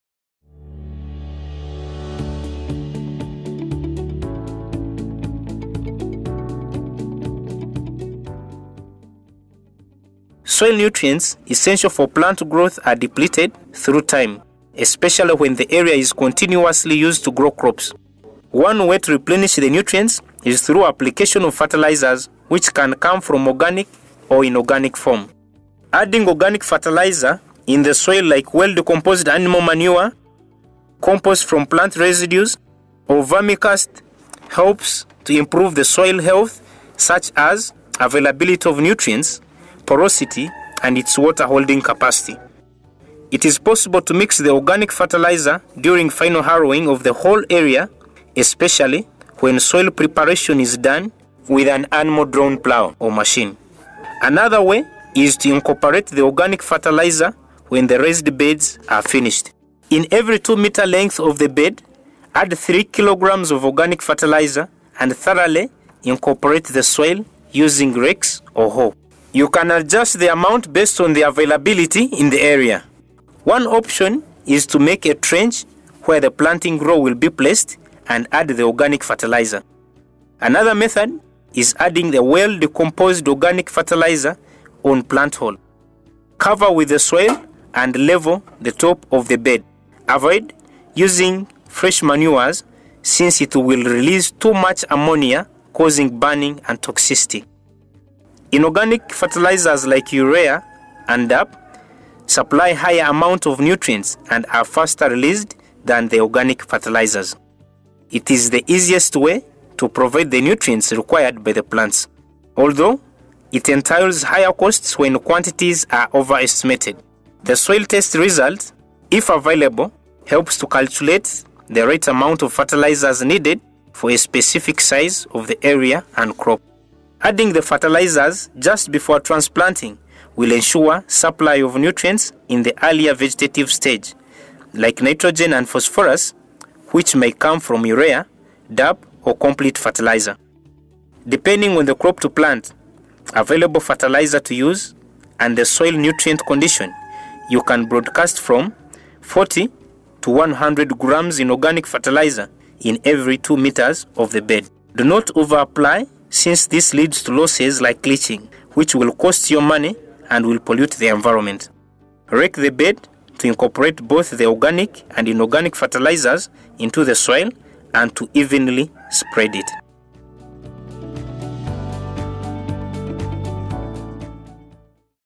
Land Preparation Part 3 – How to Apply Fertilizers to New Planting Area
With time soil nutrients are depleted. When setting up a new plot for planting season it is advised to apply fertilizers to your soil. Organic fertilizers such as well decomposed animal manure, compost from plant residues or vermicast helps to improve soil health.

Learn How To:
- Add organic fertilizers in the soil;
- Increase soil nutrients, porosity and water holding capacity;
- Measure correct quantity of fertilizer to add;
- Avoid using fresh manure;
- Add inorganic fertilizers.